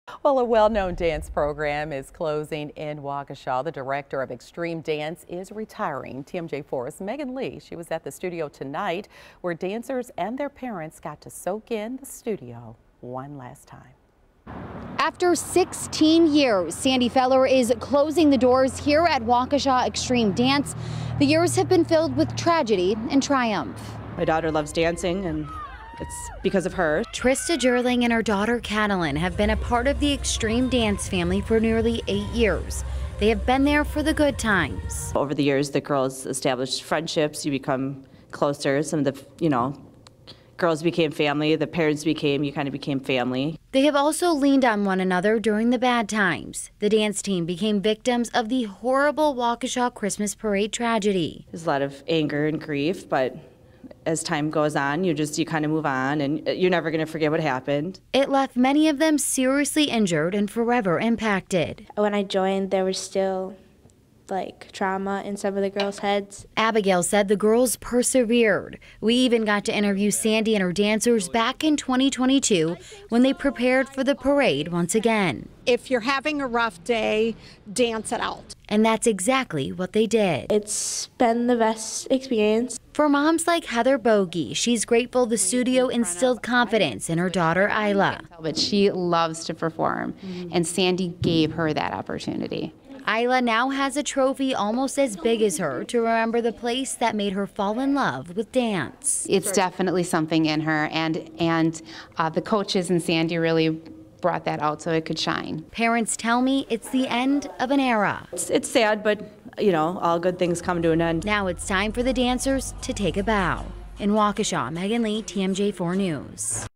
'It's been the best experience' families say goodbye to Waukesha dance studio
After sixteen years of coaching dance, Sandy Feller is closing the doors at Waukesha Xtreme Dance. The years have been filled with triumph and tragedy.

"My daughter loves to dance and it's because of her,” the mother of a dancer, Trista Jerling said.

Jerling and her daughter Katalyn have been a part of the Xtreme Dance family for nearly eight years. They have been there for many of the good times.

"Over the years the girls established friendships. You become closer. And the girls became family and the parents kind of became family,” Jerling explained.

They have also leaned on one another during the bad times. The dance team became victims of the horrible Waukesha Christmas Parade tragedy.

"It was a lot of anger and grief but as time goes on you kind of move on and you never forget what happened,” Jerling explained.

The tragic incident left many of them seriously injured and forever impacted.

[youtube.com]

[youtube.com]

On The Go with James Groh [youtube.com]